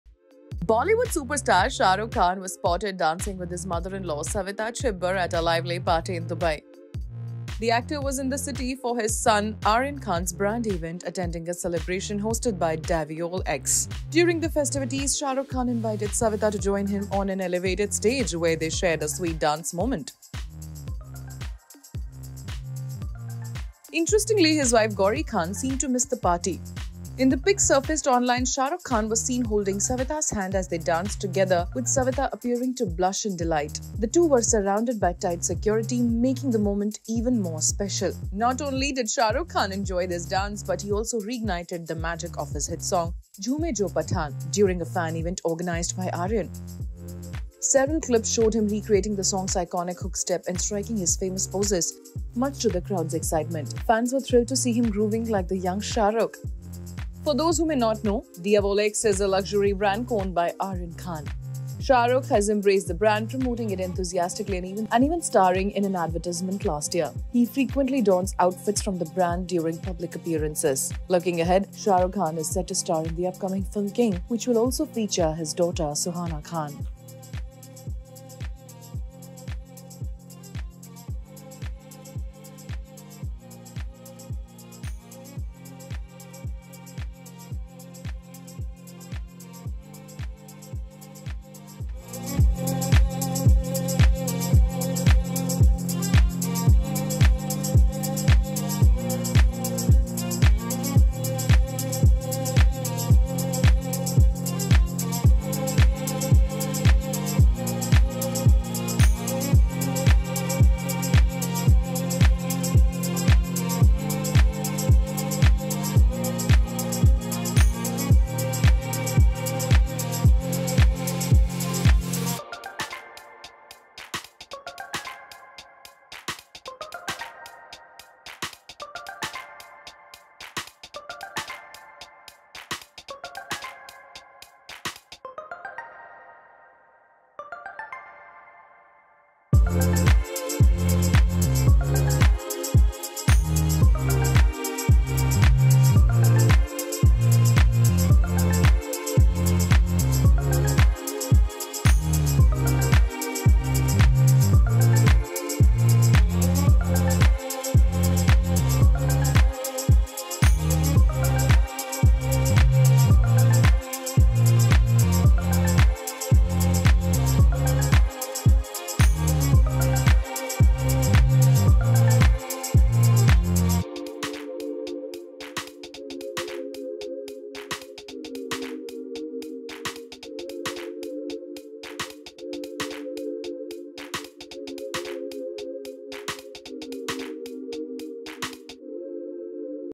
Shah Rukh Khan's CUTE POOKIE moment with his mother-in-law at a Dubai party makes fans' hearts melt
Bollywood Superstar Shah Rukh Khan was spotted dancing with his mother-in-law Savita Chibber at a lively party in Dubai. The actor was in the city for his son Aryan Khan's brand event attending a celebration. During the festivities, SRK invited Savita to join him on an elevated stage where they shared a sweet dance moment. Watch to know more!